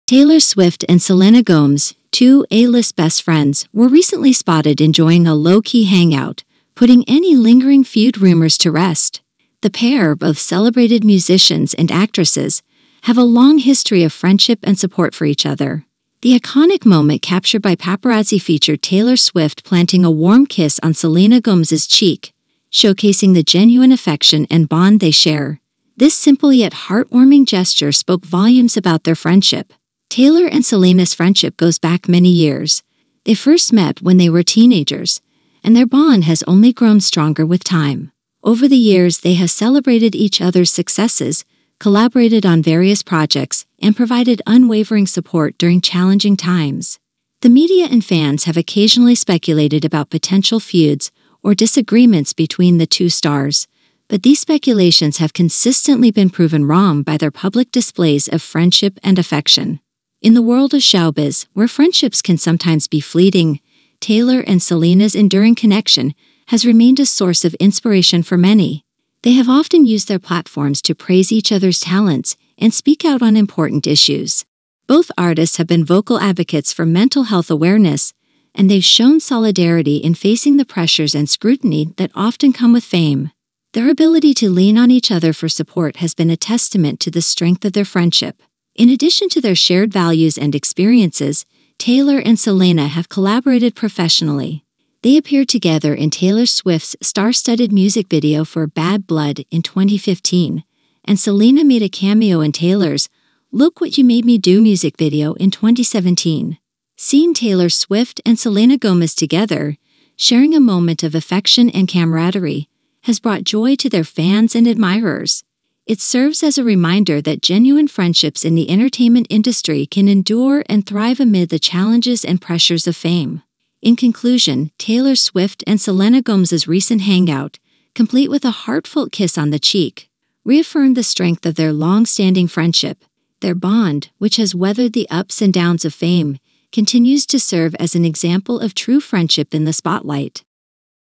Taylor Swift plants a KISS on Selena Gomez's cheek as the A list besties enjoy a low key hangout
Taylor Swift and Selena Gomez, long-standing A-list best friends, were spotted enjoying a low-key hangout, with Swift planting a friendly kiss on Gomez's cheek. Their heartwarming display of friendship warms the hearts of fans, highlighting the enduring bond between these two megastars. The duo's close relationship serves as a reminder of the importance of genuine friendships in the world of entertainment.